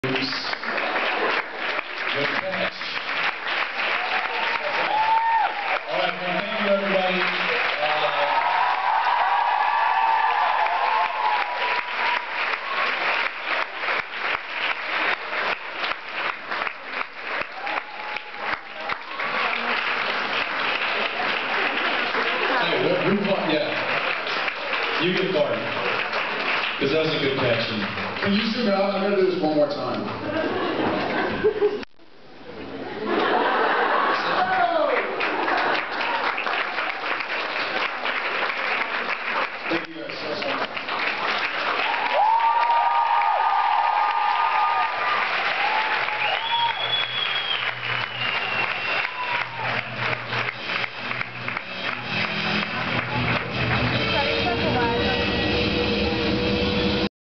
Supernatural JIB Con 2011 - Jared and Jensen panel the end. (09.april)
My JIB Convention video - Saját Supernatural JIB con 2011 videó, ne nyúld le. Jared és Jensen panel vége.